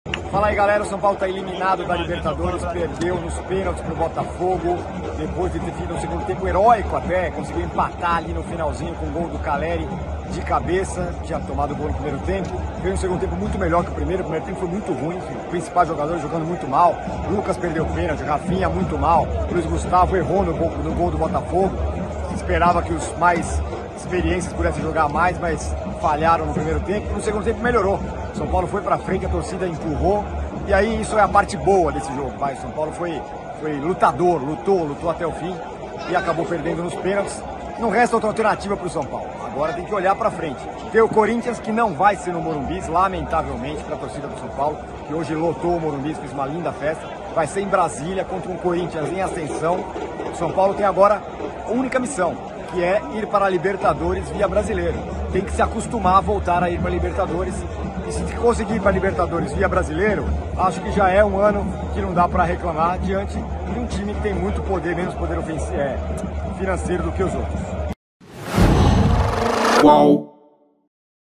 SÃO PAULO ELIMINADO! 'LUCAS, LUIZ GUSTAVO, RAFINHA... OS MAIS EXPERIENTES FALHARAM', DIZ TIRONI
Em jogo dramático, Botafogo eliminou o São Paulo nos pênaltis, nesta quarta-feira (25), nas quartas de finais da Libertadores. Para Tironi, o tricolor paulista foi lutador. O foco agora é voltar à Libertadores via Brasileirão.